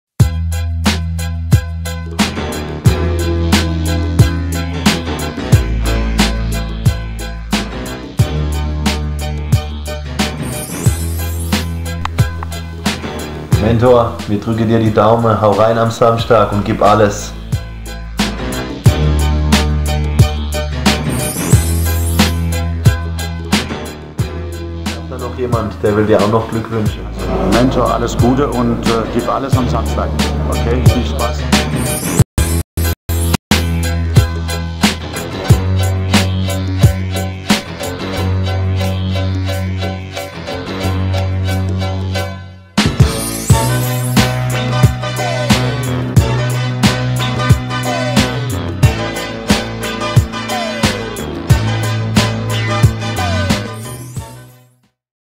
LUAN KRASNIQI und Claudio grüßen MENTOR KRASNIQI
Luan und Claudio wünschen Mentor viel Glück für den Fight am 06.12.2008